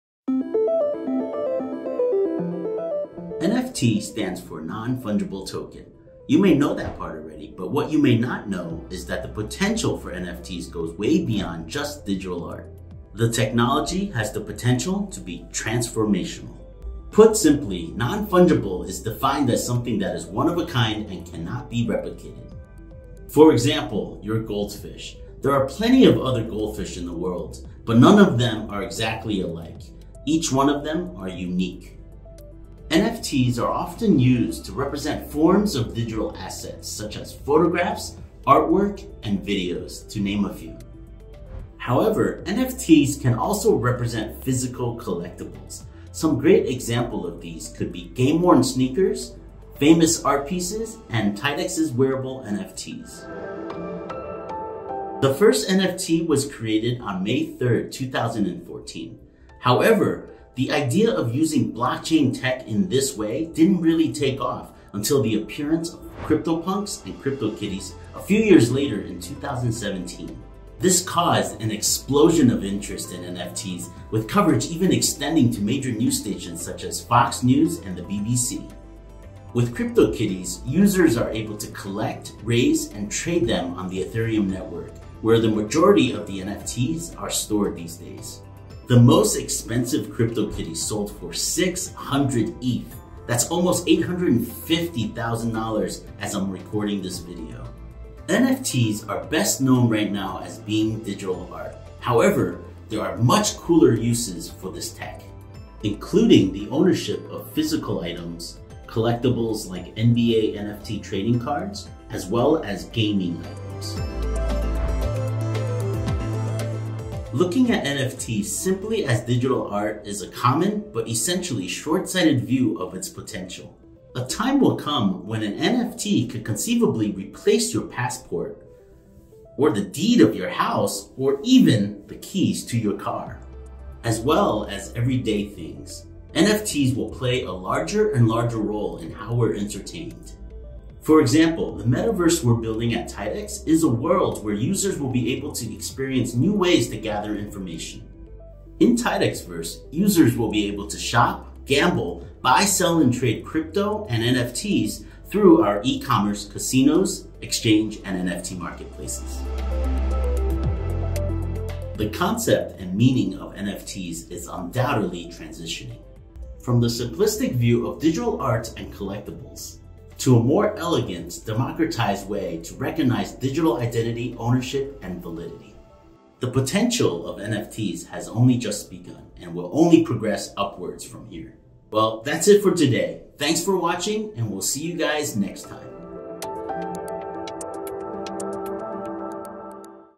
What are NFTs?
NFT stands for non-fungible token - you may already know that part, but what does that mean, exactly?

In this video, TIDEX CEO, Eric Ma, breaks down the origins of NFTs and what they could be used for in the future…

Sign up, make your first deposit and start trading today